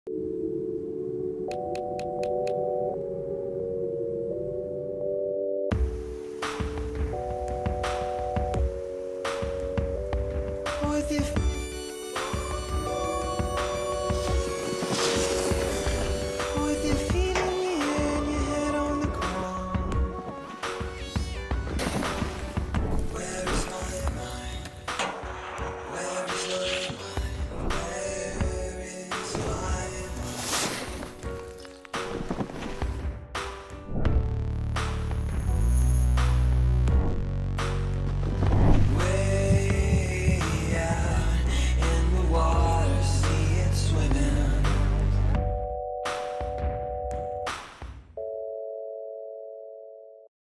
ESBEGINNT - Stubai Zoo | Freeski Movie | Slopestyle
Check the Video from our yesterdays Park Session at Stubai Zoo we did with SportScheck.